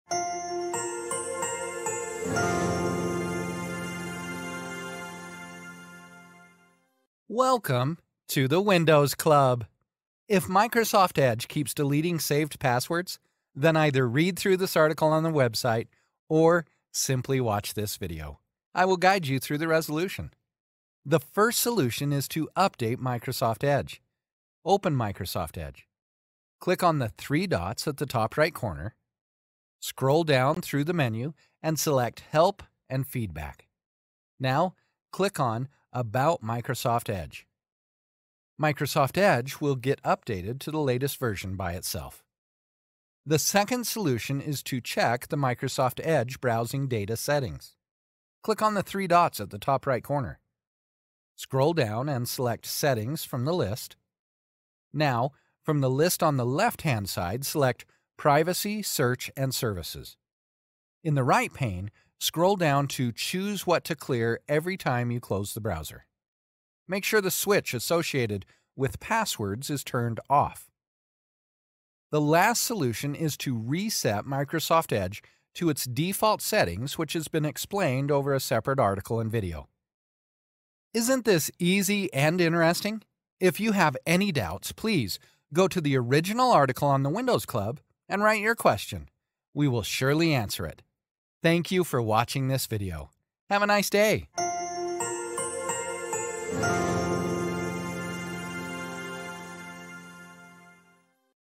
Microsoft Edge keeps deleting saved passwords
Microsoft Edge is one of the best web browsers for Windows users. It comes with some unique features that you will not find in other web browsers, like Math Solver, Vertical Tabs, etc. If you are a Microsoft Edge user, you probably know that Edge lets you save passwords for your favorite websites just like other web browsers. For some users, this feature is not working as expected. According to them, Edge keeps deleting saved passwords. Every time they close Edge, their saved passwords get deleted automatically.

If you are also experiencing the same problem in Edge, the solutions provided in this tutorial may help you.

1] Update Microsoft Edge

2] Check the Clear browsing data setting

3] Reset Microsoft Edge settings to default

4] Uninstall and reinstall Edge

Timecodes:
0:00 Intro
0:22 Update Microsoft Edge
0:43 Check Microsoft Edge browsing data settings
1:14 Reset Microsoft Edge